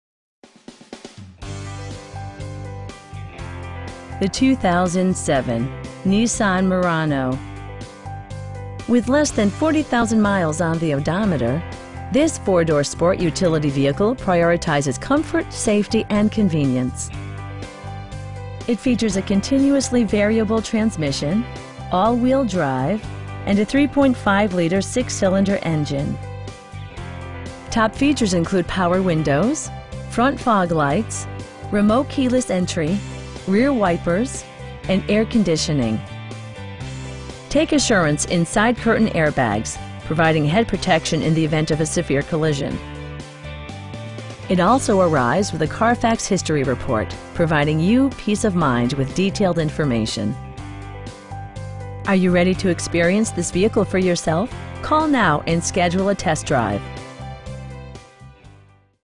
2007 Nissan Murano SE JUST TRADED NEW TIRES
Sheehy Nissan - Manassas
9010 Liberia Avenue
in Manassas, VA 20110

The 2007 Nissan Murano. With less than 40,000 miles on the odometer, this 4 door sport utility vehicle prioritizes comfort, safety and convenience. It features a continuously variable transmission, all-wheel drive, and a 3.5 liter 6 cylinder engine. Top features include power windows, front bucket seats, front fog lights, tilt steering wheel, power door mirrors, remote keyless entry, rear wipers, and air conditioning. Take assurance in side curtain airbags, providing head protection in the event of a severe collision. It also arrives with a Carfax history report, providing you peace of mind with detailed information. Are you ready to experience this vehicle for yourself?